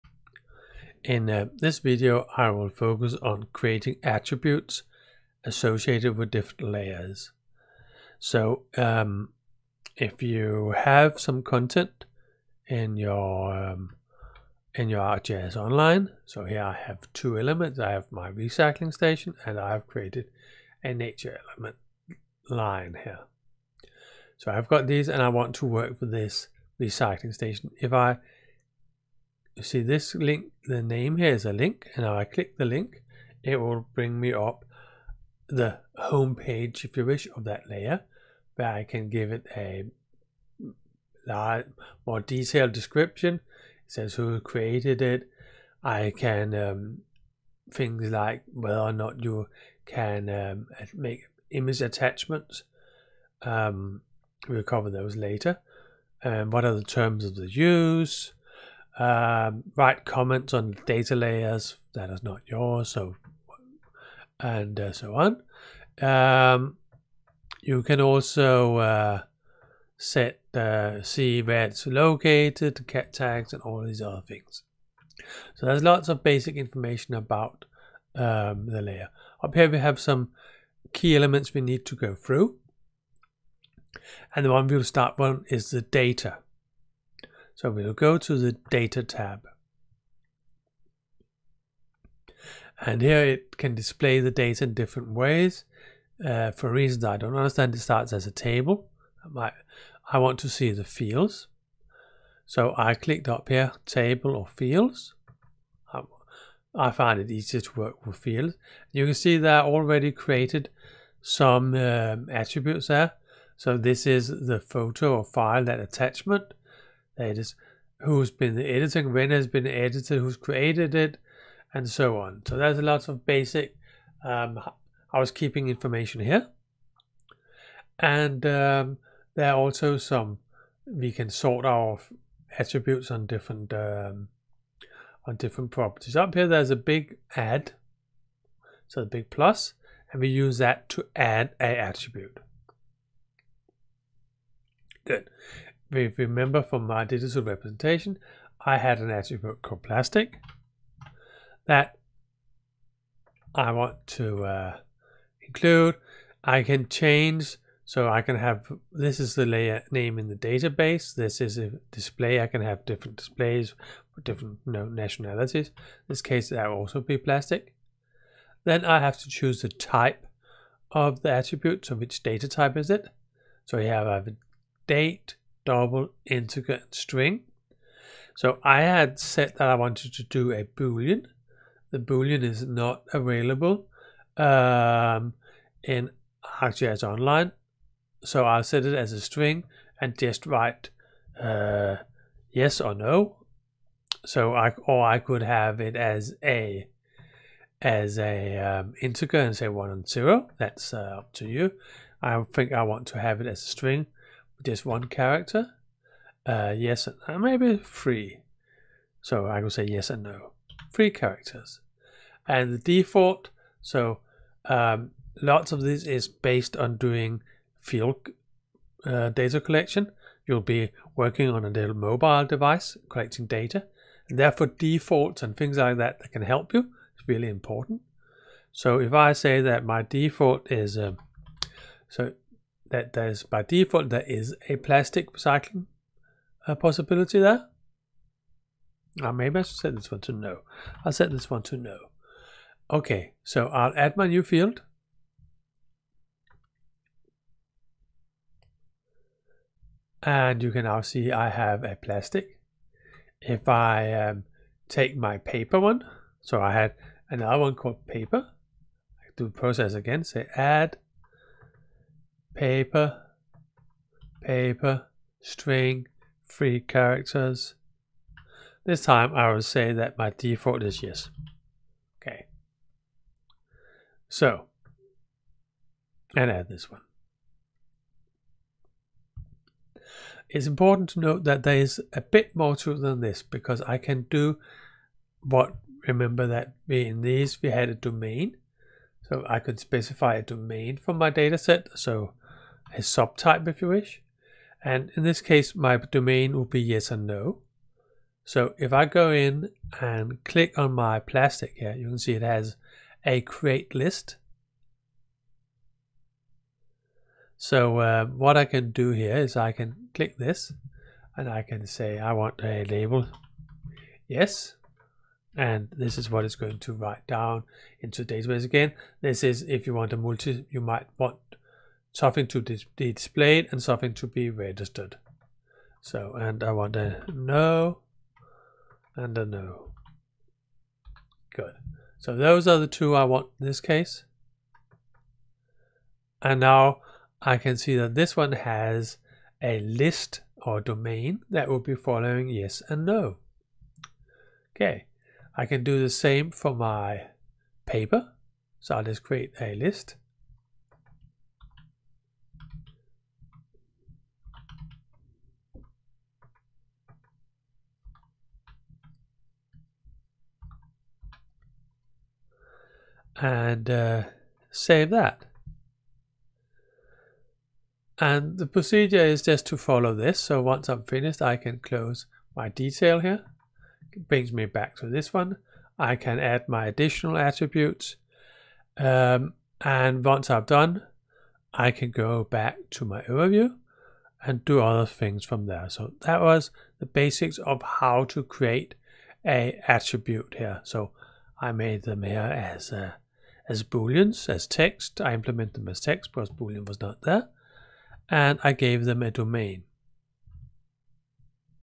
ArcGIS online episode 6 adding attributes
0:00 Introduction
0:38 Setting the properties of a layer
1:26 Open the data tab
2:16 Add an attribute.
04:10 Do it again.
04:40 Set a Domain on an attribute (list of values)
6:23 Getting back to the property page